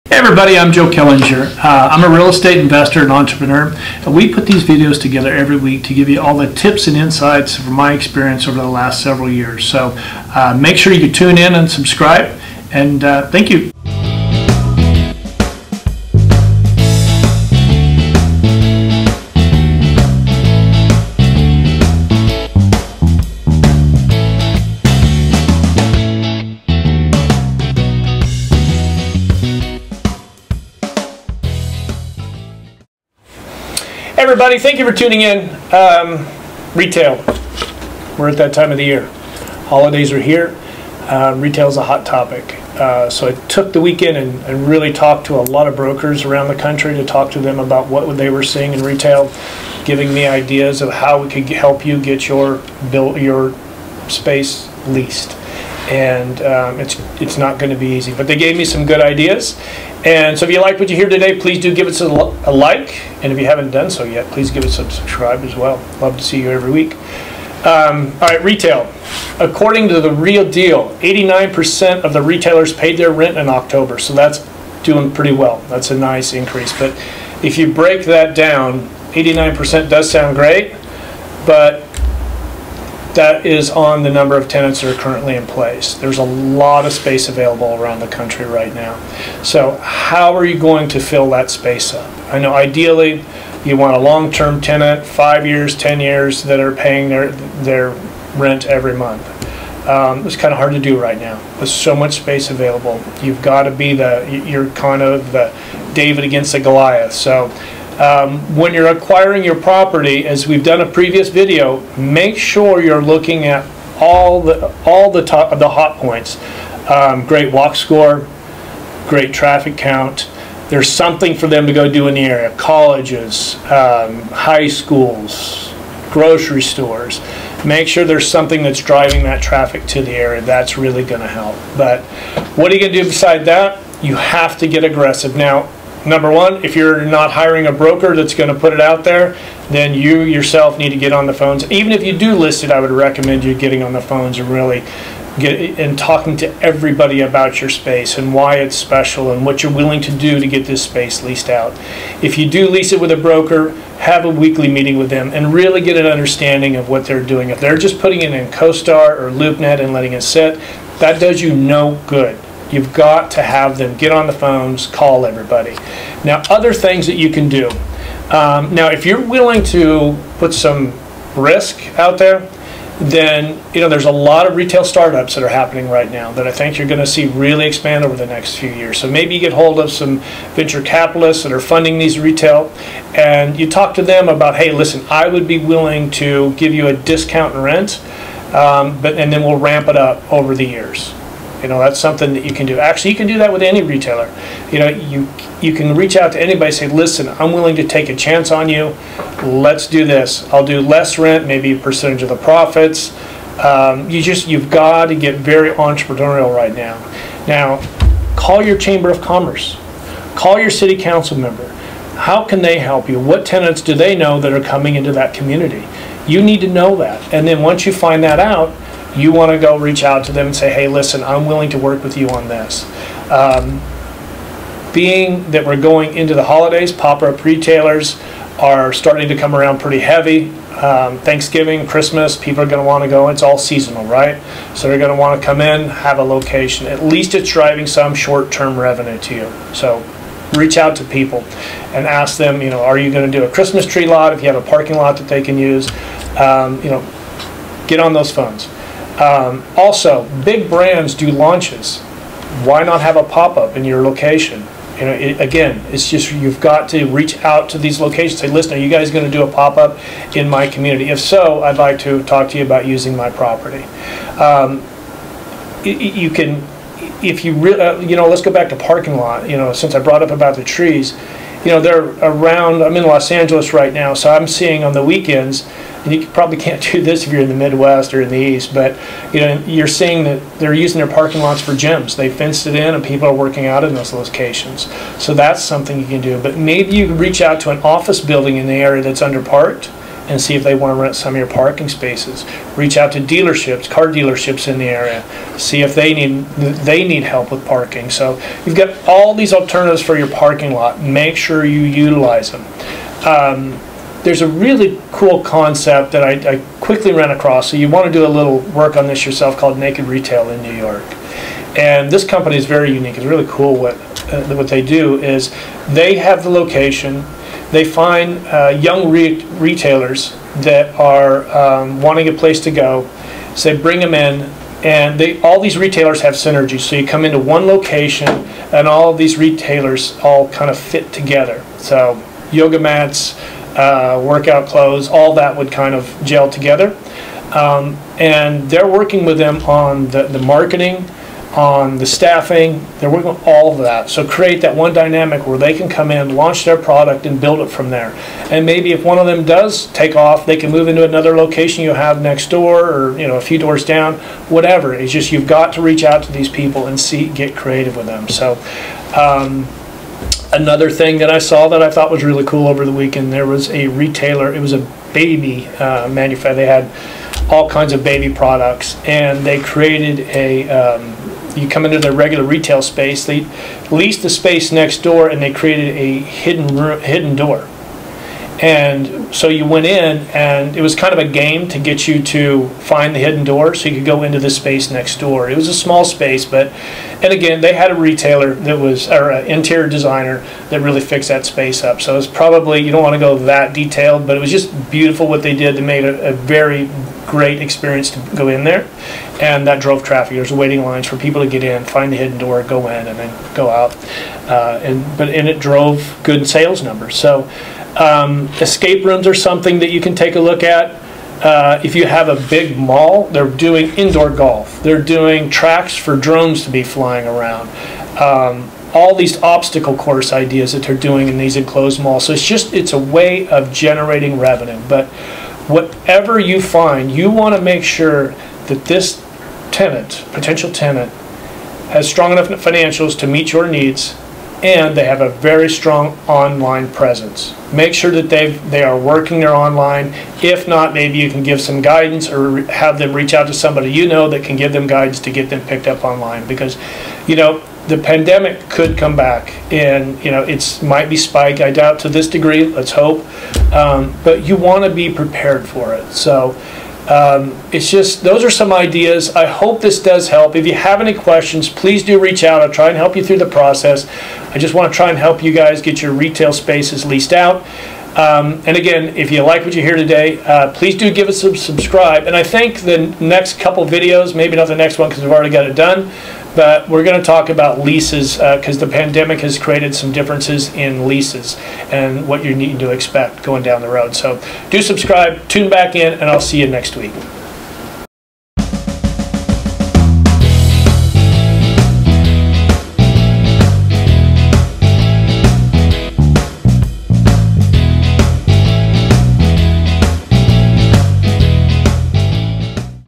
All You Need To Know About Leasing Your Retail Space
This week, Joe Killinger discusses what you need to know about leasing your retail space and using it to the fullest.